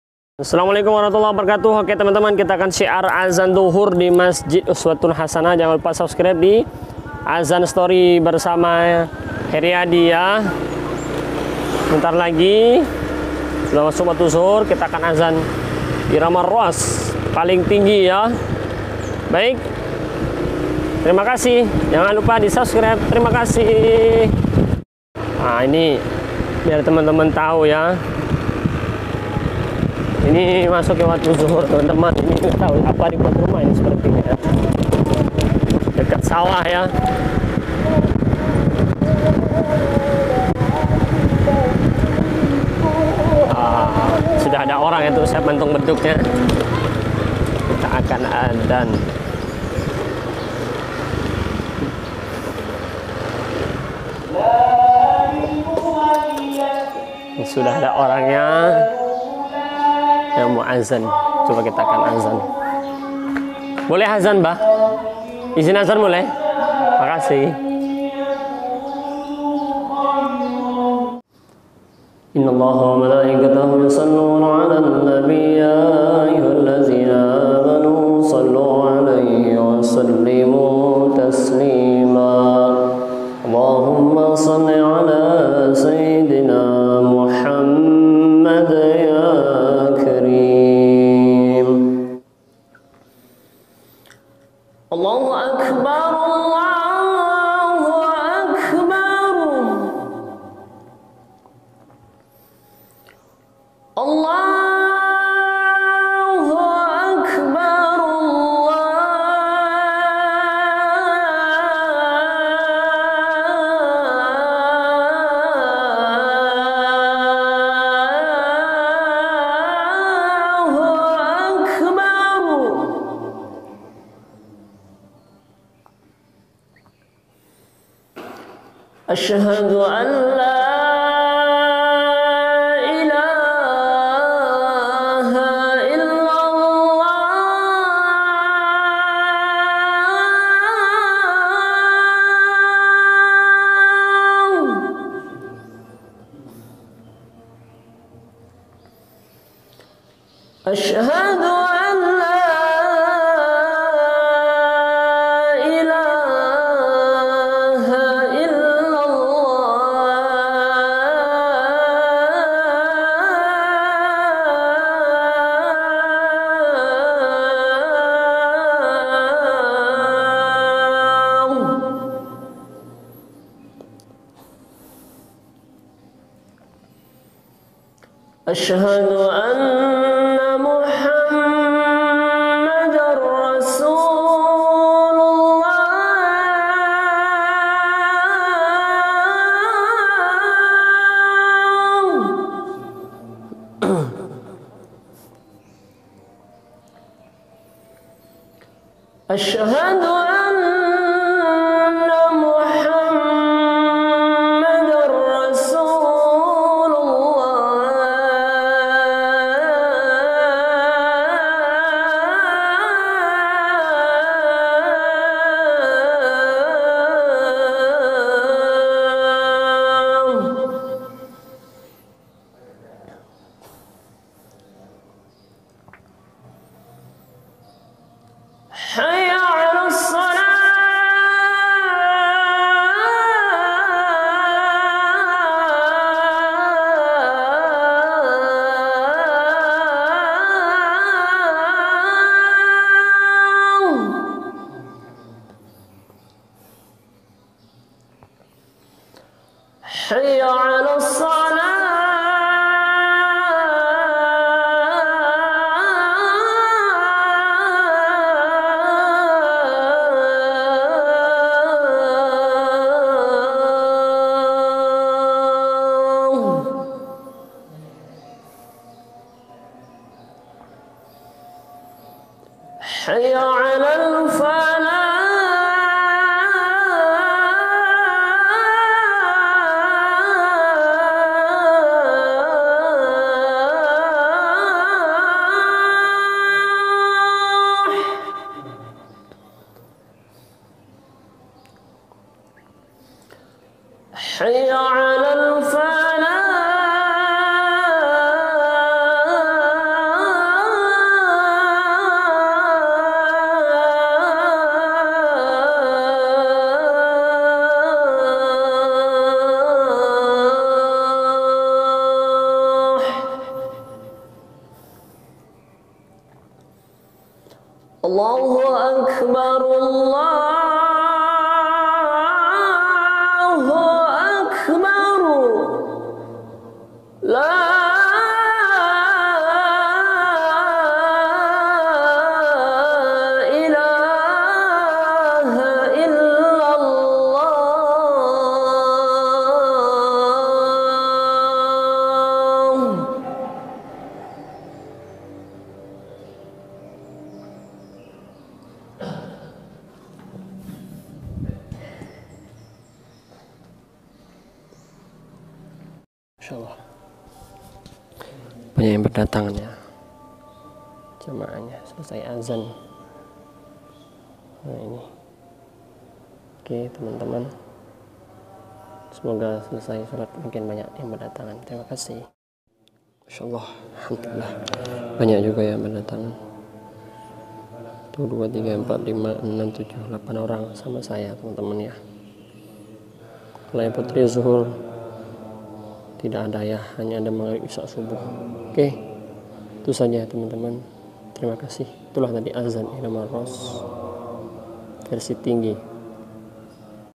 Heboh! Adzan Dzuhur Paling Dinantikan Jamaah Irama Rost Tertinggi Bapake Penasaran Khas Indonesia
Mencoba nekat Adzan Rost paling Tertinggi khas orang Indonesia pada umumnya langsung Ramai Berdatangan Jamaah.